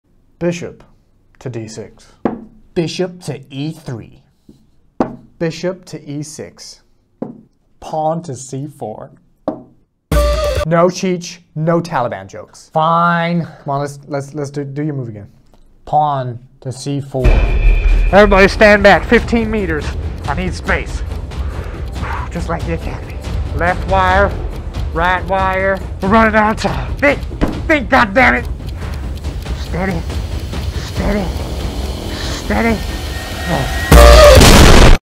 we're not making a Taliban joke
at first Cic wasn't a fan of this sketch, but then he said it was "pretty funny" so I guess we'll post it?

we made one sketch a day for 24 days